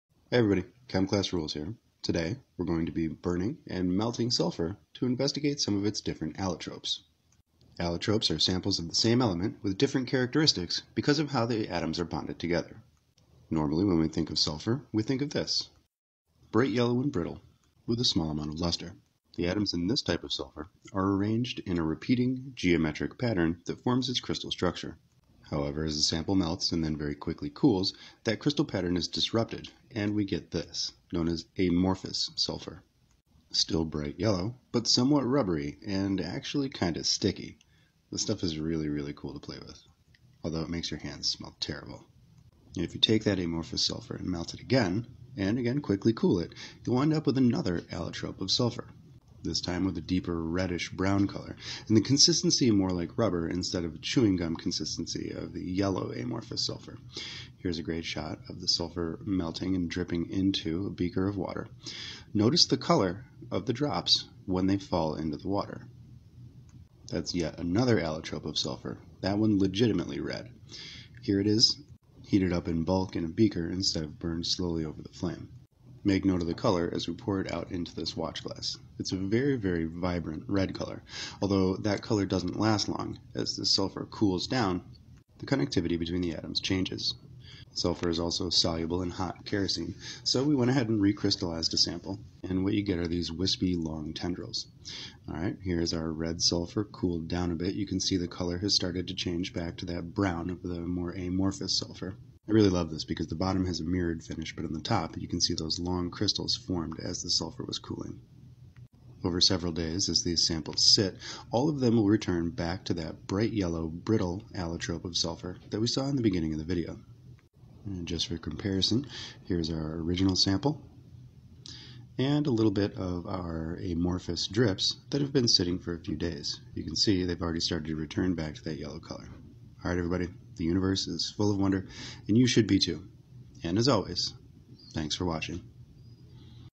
Burning sulfur to investigate a few of it's allotropes (red is my favorite)
"Many of the allotropes of sulfur can be studied using this simple experimental apparatus. An initial sample is loaded into the set up and burned over a beaker of cool water. Each sample is then collected and burned again revealing this elements beauty and luster"

What did you do in school today honey? We learned about fire an brimstone. What's not to love about this demo? Fire, color changes, back and forth phase changes, amorphous gooey semi-solids, it's got it all.

teachers note - do this in the fume hood. you will be generating sulfur dioxide gas, which you don't want you or your students near. you also don't want to have to explain why the school smells like raw sewage. so definitely a fume hood demo. save this demo set-up, it comes in crazy handy for lots of things, like burning food to show it's energy density / fat content. (you're a good teacher, keep it up, the mess is worth it, you got this and when you don't it's ok)